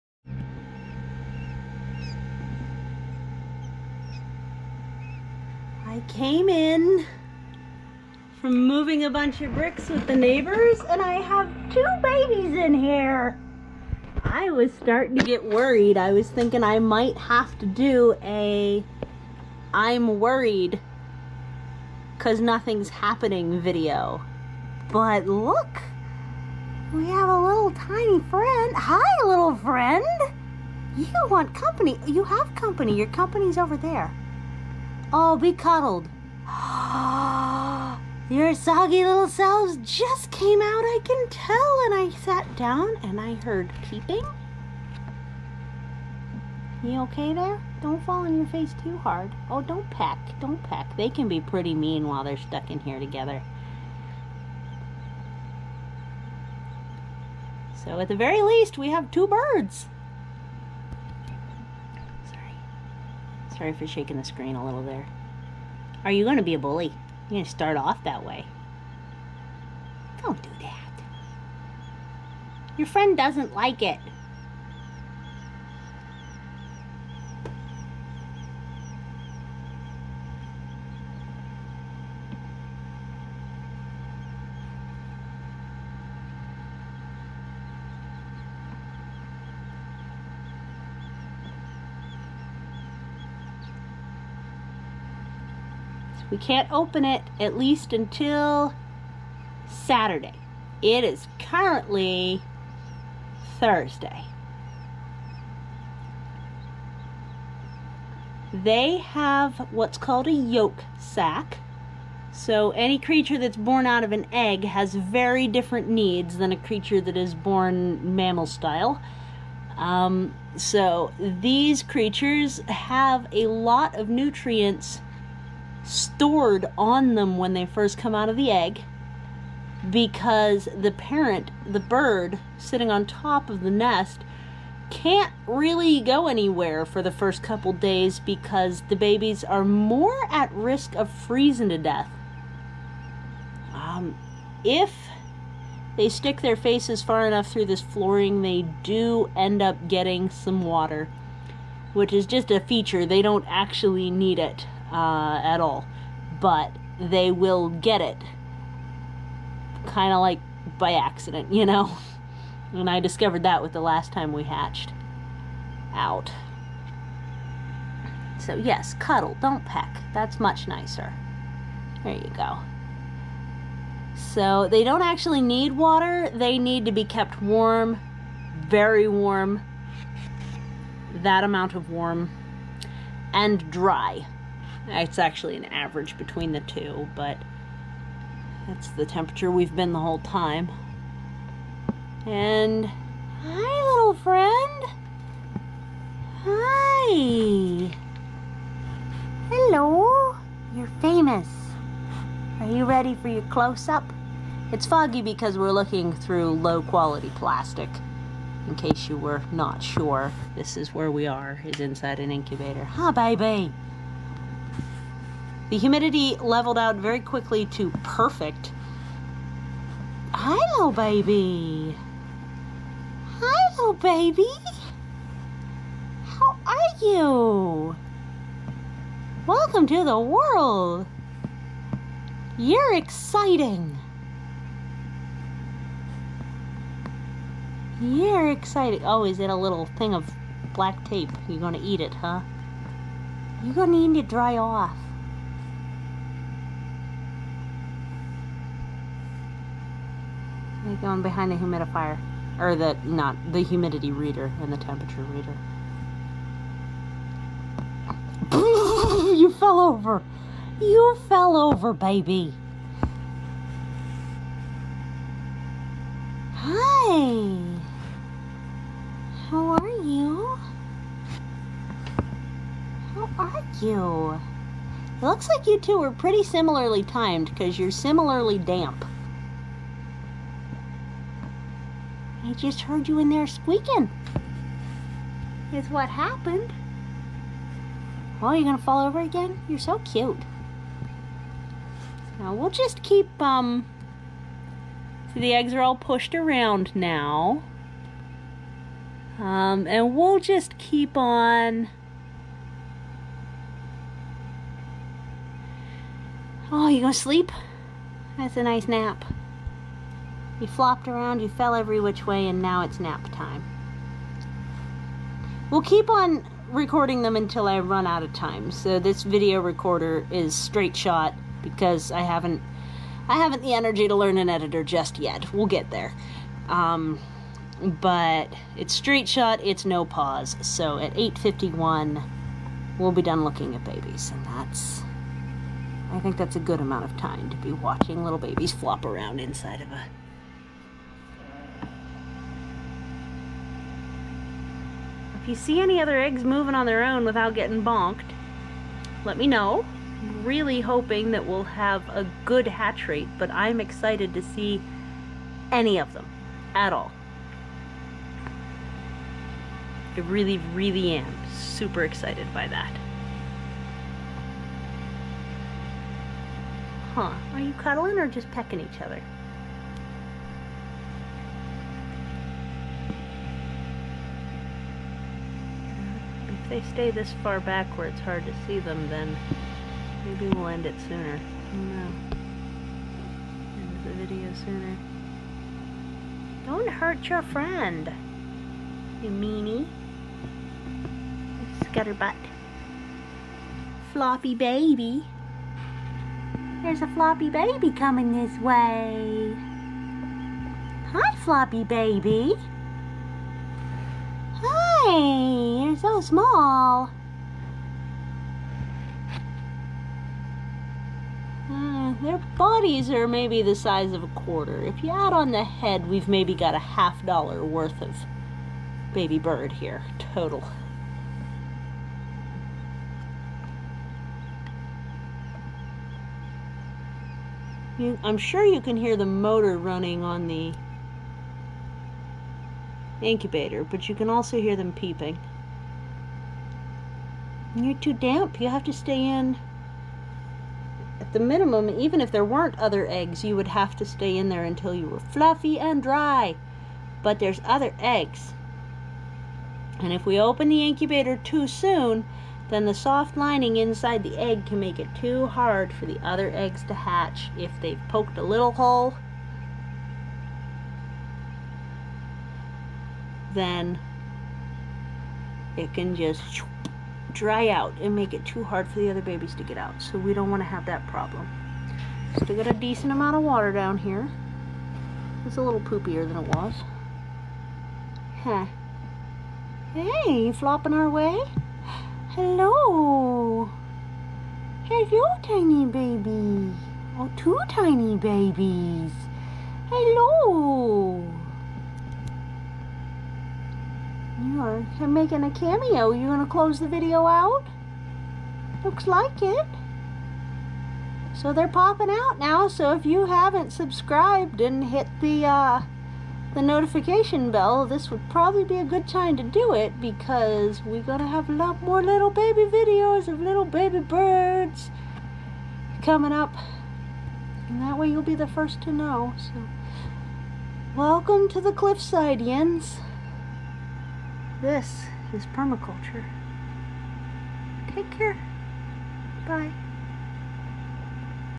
First BABIES of this HATCH - 2 baby quail damp and peeping
The incubator is alive with activity now, it's so exciting!